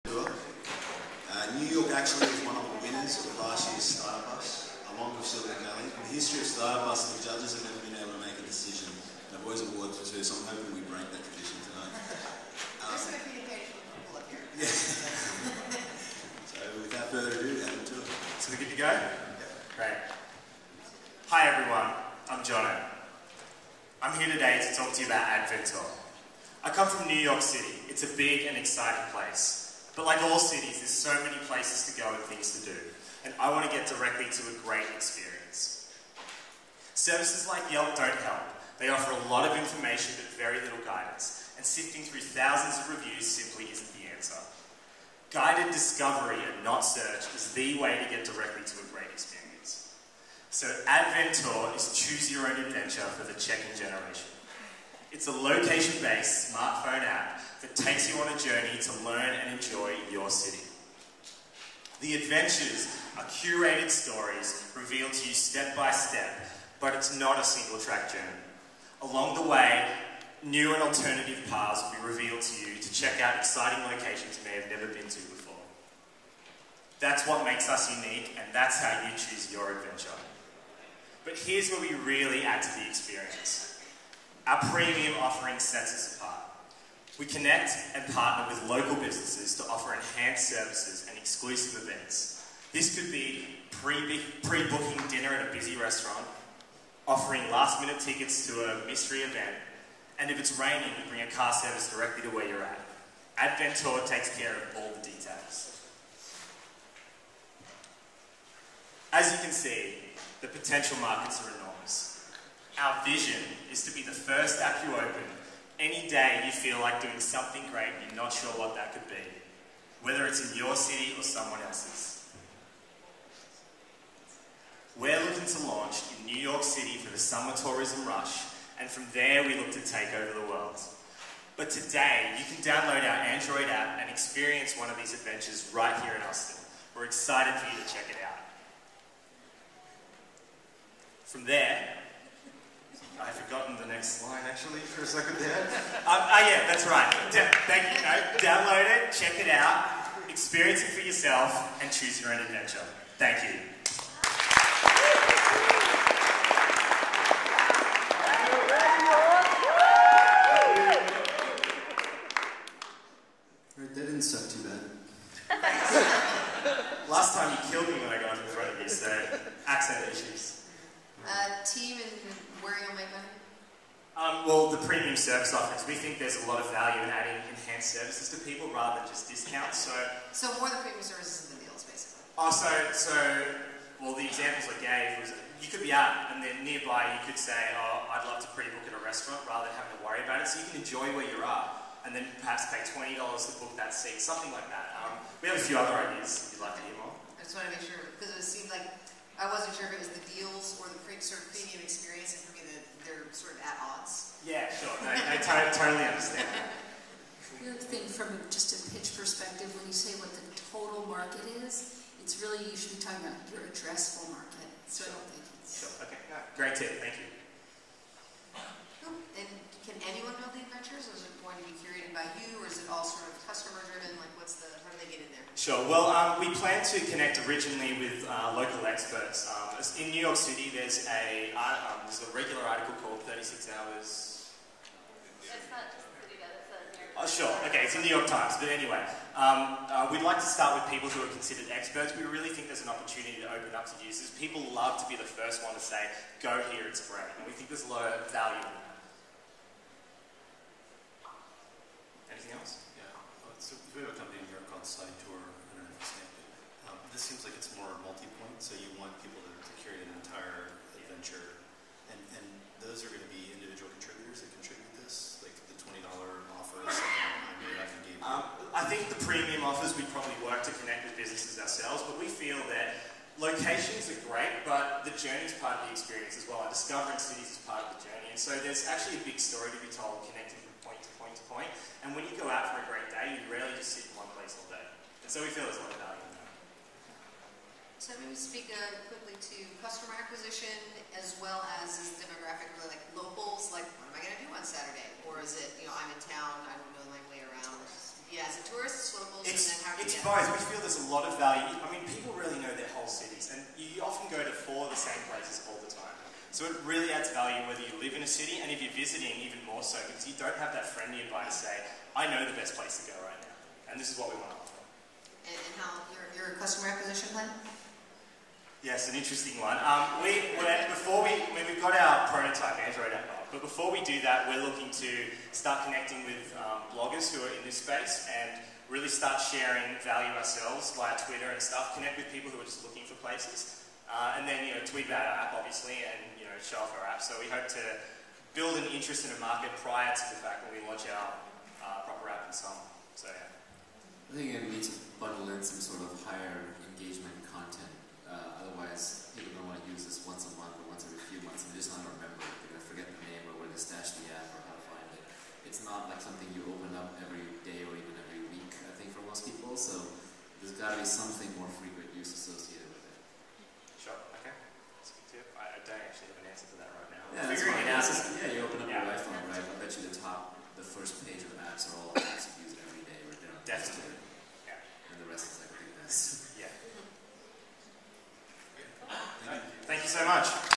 Startup Bus 2012 Adventeur
Filmed during the Startup Bus 2012 final pitch contest, live at SXSW 2012.